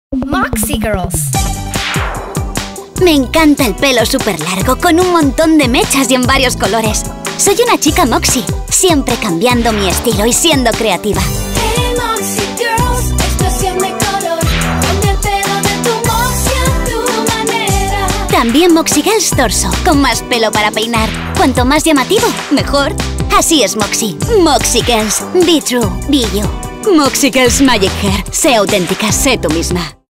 Moxie Girlz Magic Hair and Makeover Styling Head Dolls Commercial (Spanish version, 2009)
Moxie Girlz Magic Hair. Se auténtica! Se tu misma!